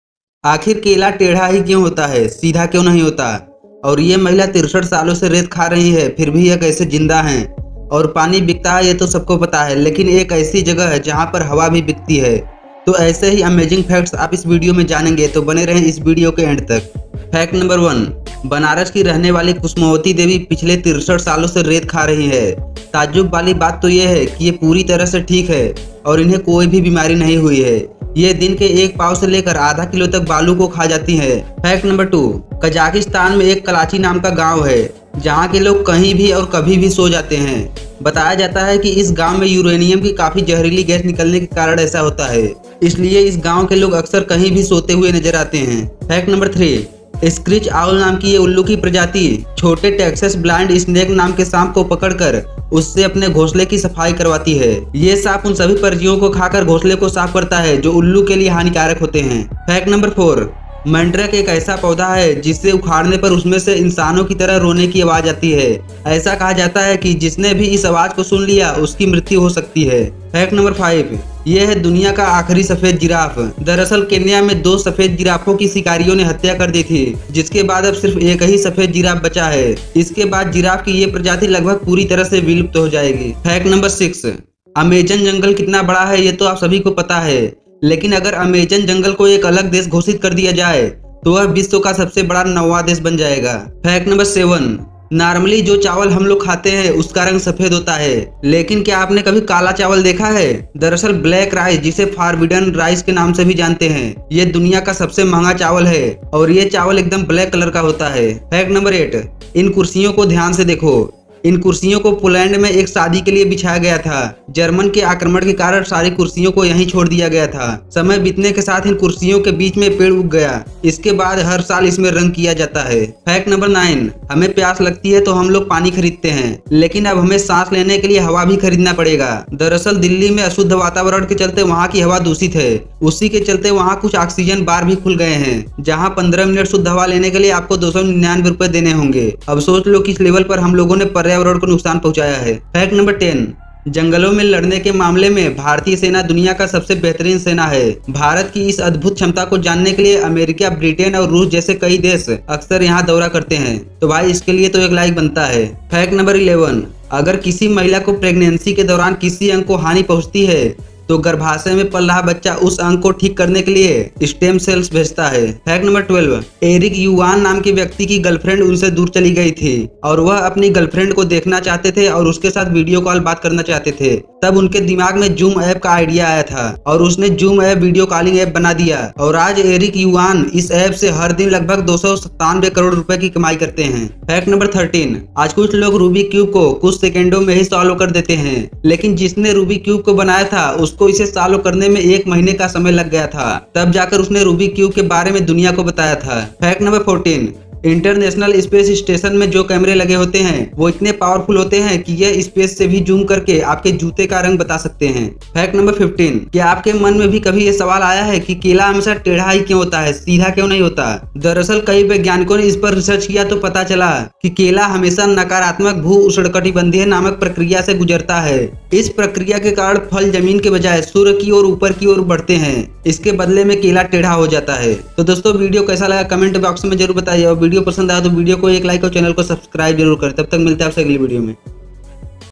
केला टेढ़ा ही क्यों होता है ? सीधा क्यों नही होता | Amazing Facts | 15 Random facts
about  this video :- hello dosto is video me maine bahut hi amazing aur interesting facts ke bare me bataya hu ummid hai ye video apko pasand ayegi

best mic for YouTube

Nonprofit educational or personal use tips the balance